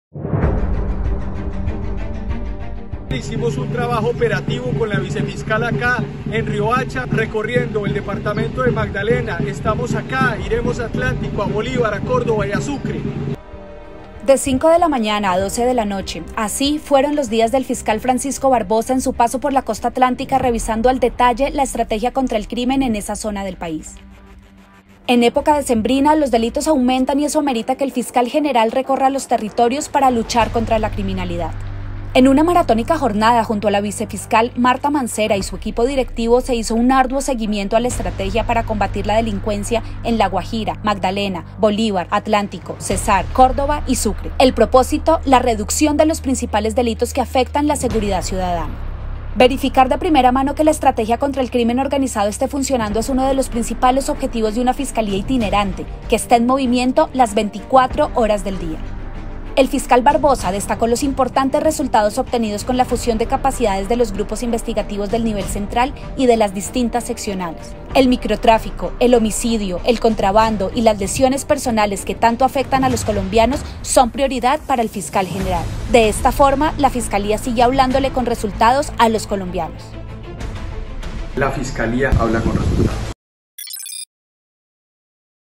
Fiscal Francisco Barbosa en la Costa Atlántica, así fue la maratónica jornada
De 5 a.m. a 12 de la noche, así fueron los días del Fiscal Barbosa en su paso por la Costa Atlántica revisando la estrategia contra el crimen. En diciembre los delitos aumentan y eso amerita que el Fiscal recorra territorios para luchar contra la criminalidad.